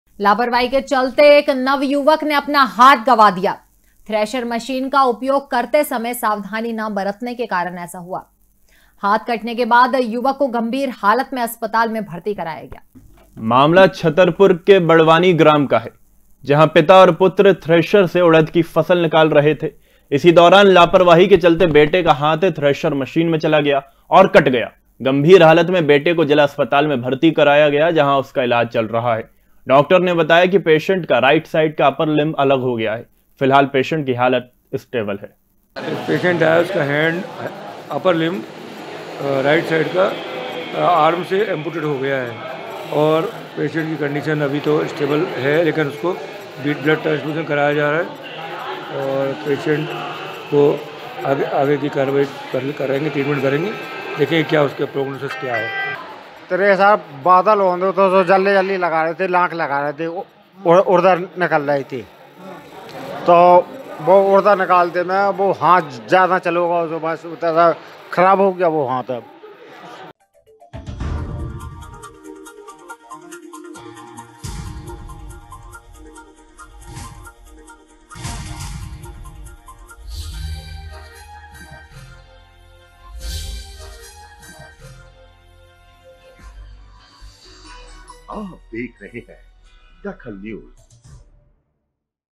DAKHAL NEWS लापरवाही के चलते थ्रेसर मशीन से कटा हाथ
लापरवाही के चलते थ्रेसर मशीन से कटा हाथ
गंभीर हालत में जिला अस्पताल में कराया भर्ती